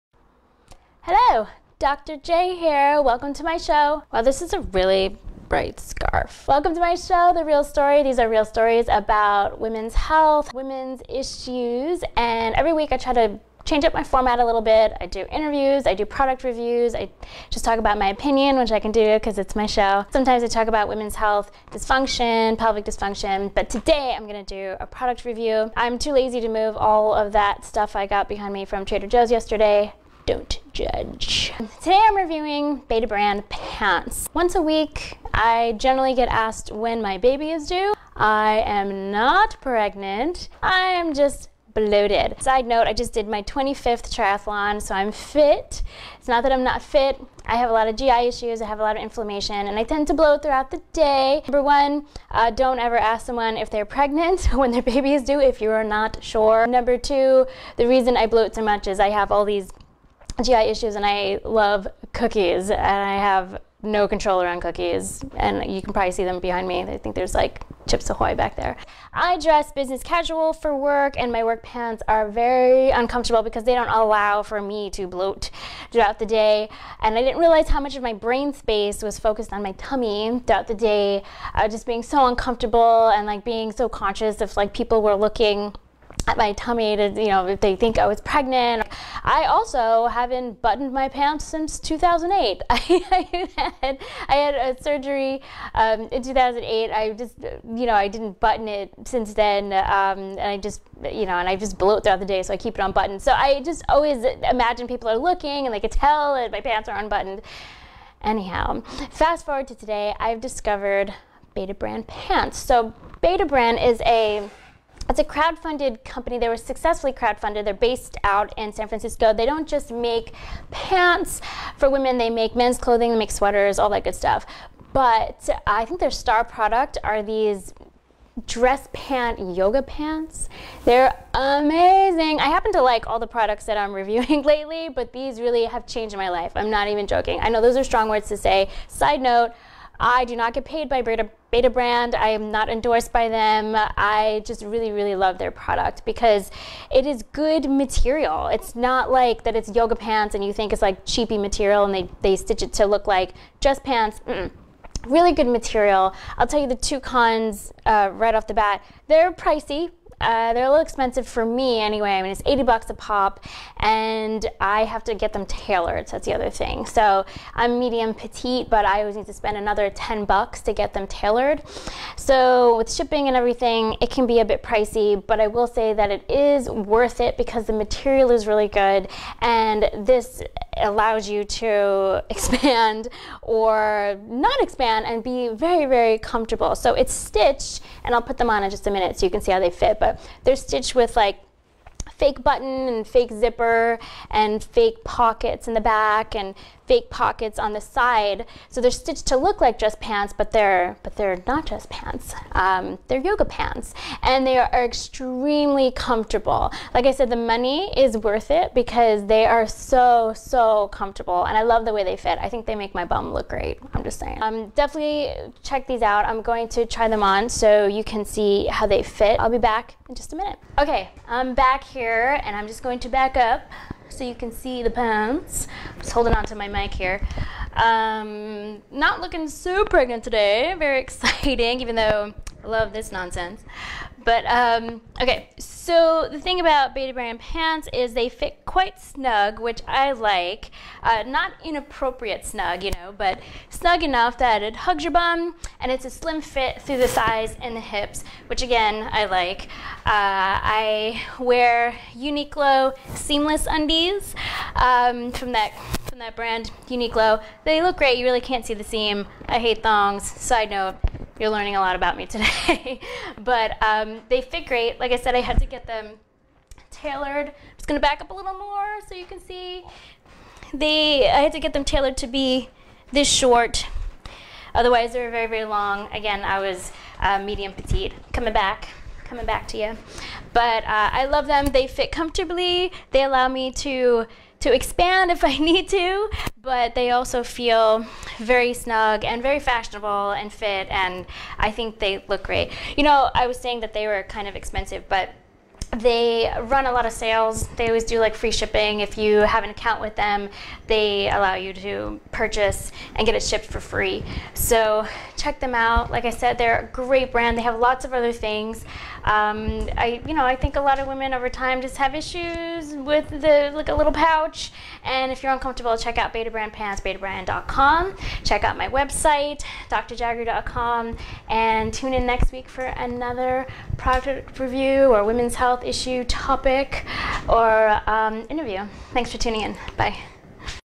Betabrand Dress Pant Yoga Pants Review: The Real Story
Denise Jagroo, women's health specialist, reviews Betabrand Dress Pant Yoga Pants.